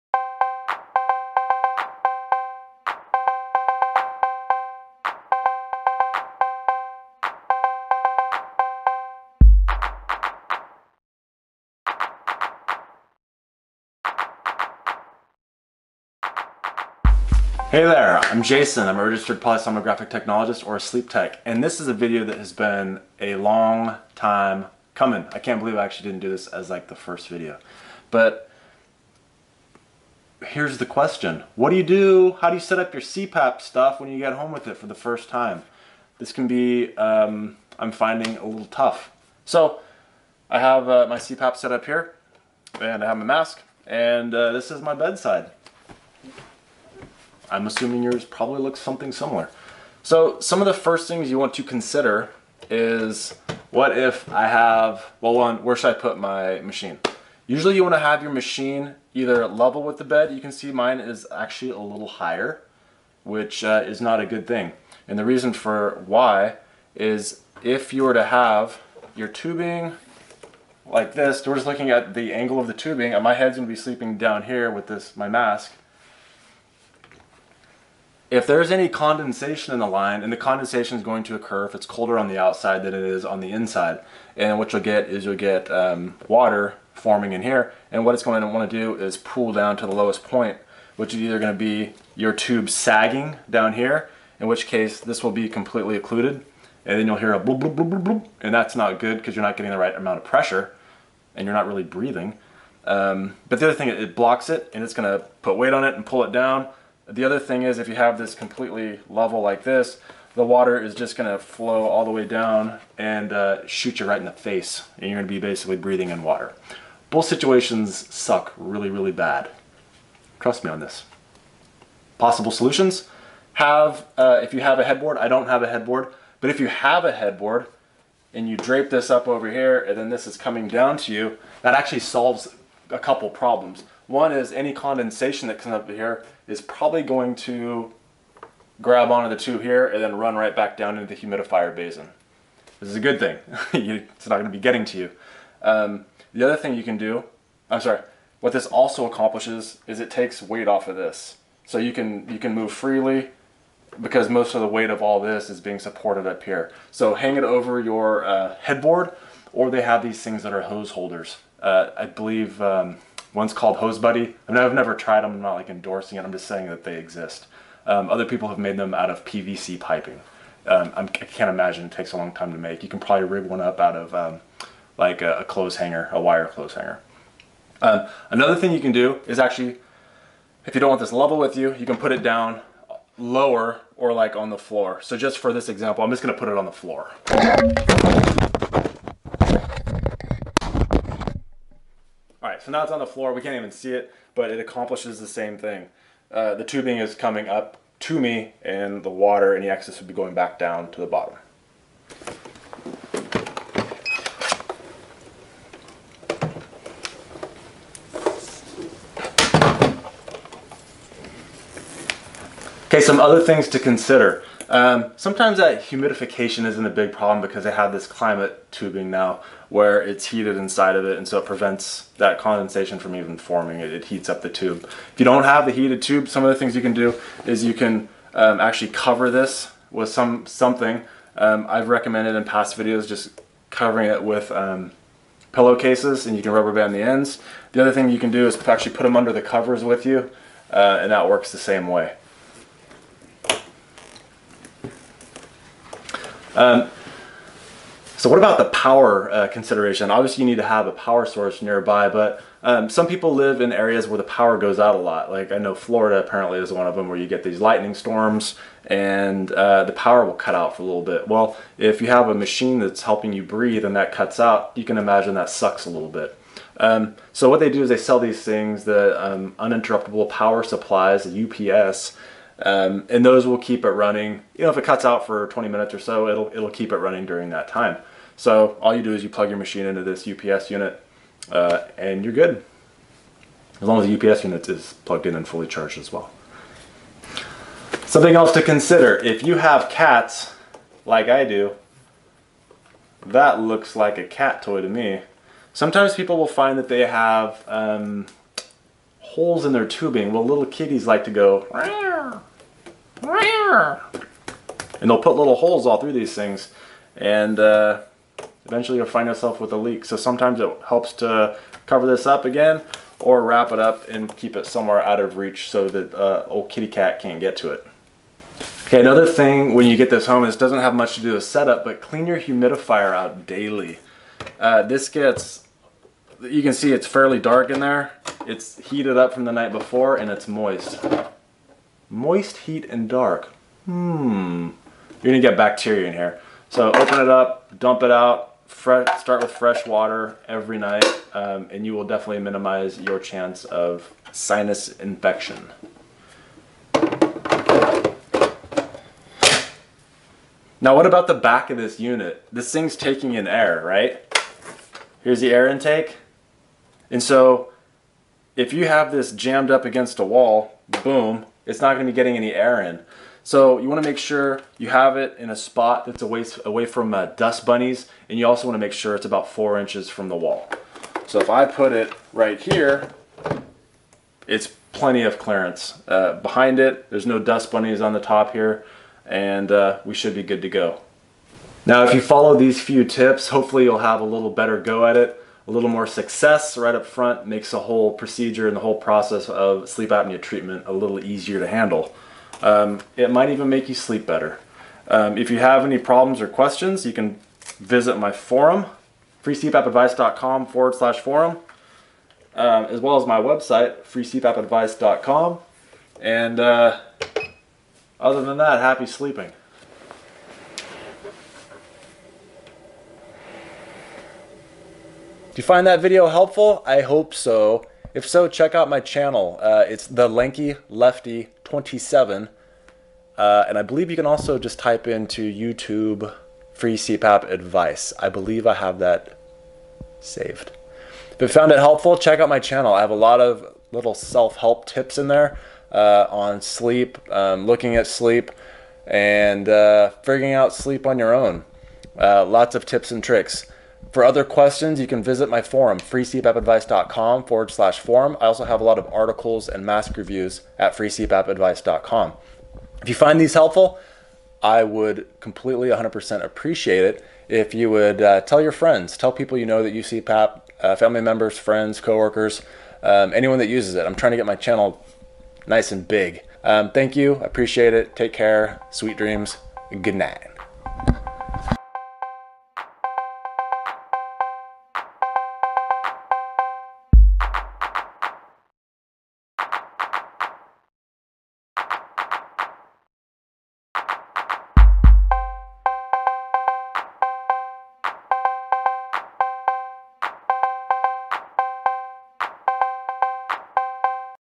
Home CPAP Set up for New CPAP Therapy Users. CPAP in the Bedroom
Join the FreeCPAPAdvice.com/forum Online CPAP Support Community.

New to CPAP?  This video discusses how to set up your CPAP machine in your room.  Tips and tricks are discussed.